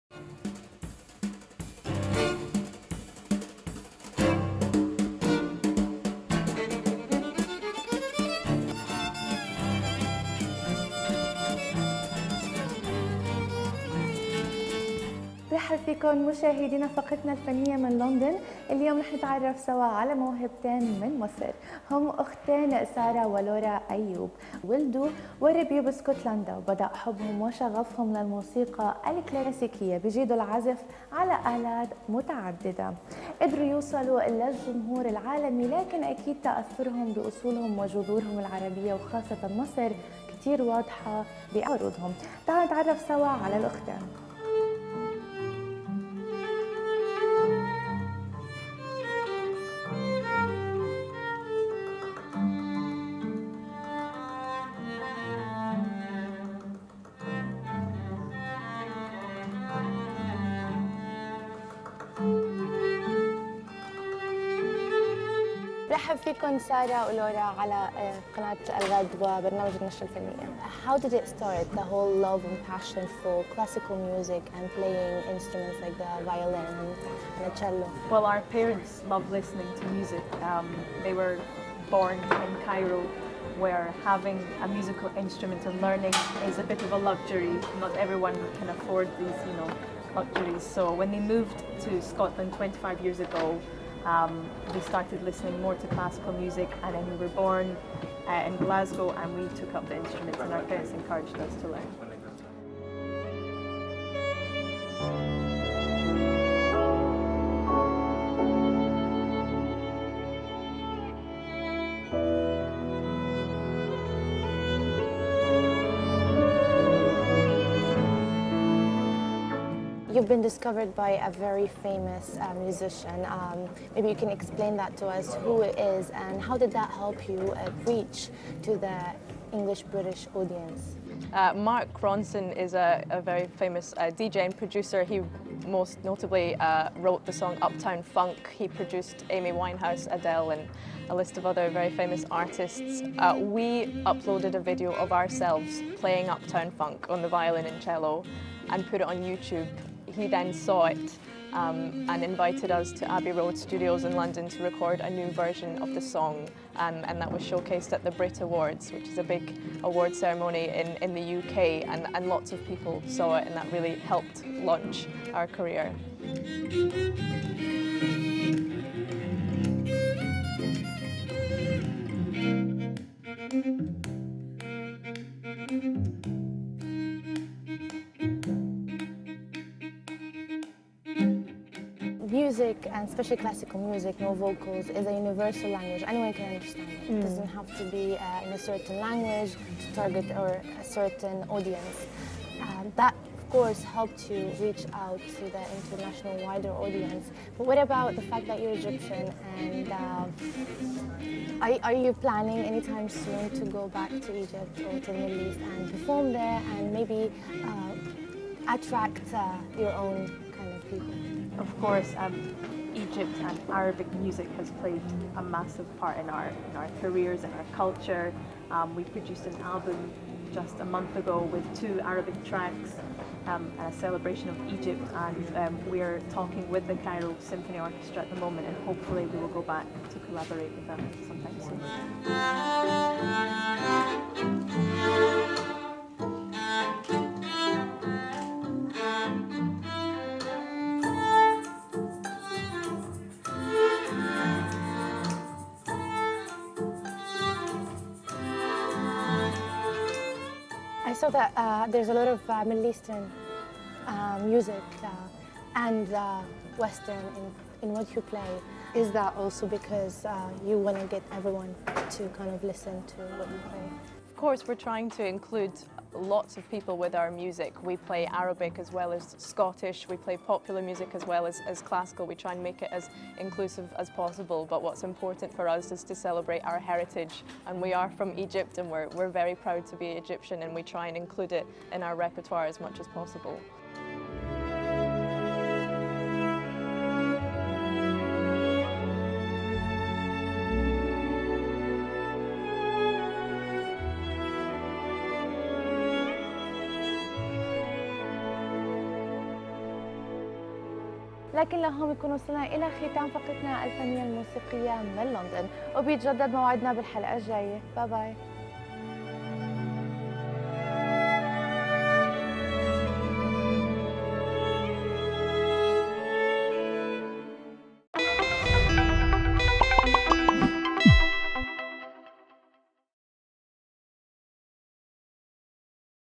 النشرة الفنية | موسيقيتان مصريتان تجمعان بين الموسيقي الغربية والإيقاعات العربية
حفل "الأختان أيوب" في لندن، لارا و سارة أيوب موسيقيتان مصريتان تجمعان بين الموسيقي الغربية الكلاسيكية والإيقاعات العربية، تحاورهما لارا سمرا،  في لقاء خاص للنشرة الفنية